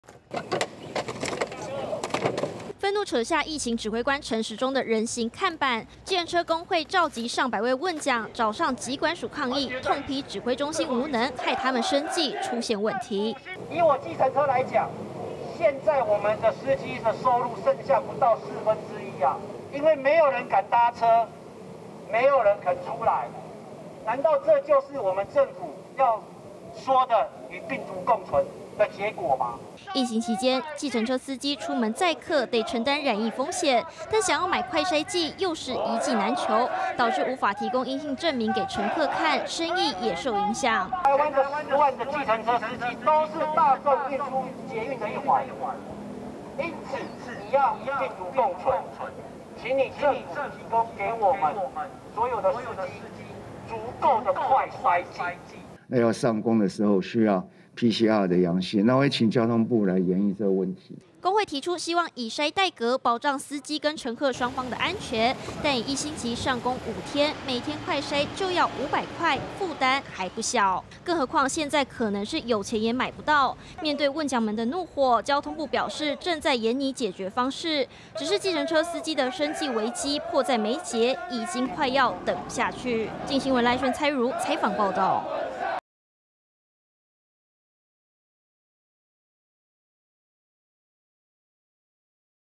承擔染疫高風險 運將盼政府提供快篩防疫｜鏡新聞
上百名計程車司機，今天(13)前往疾管署抗議，控訴指揮中心沒有規劃好防疫政策，讓他們生計受影響。計程車職業工會表示，司機搭戴客人要承擔染疫風險，乘客也會希望司機出示快篩陰性證明才搭車。但運將，一，是根本搶不到快篩劑，二，是如果要每天快篩，快篩劑費用是一筆負擔。工會強調，計程車也是大眾運輸的一環，希望指揮中心能協助解決問題。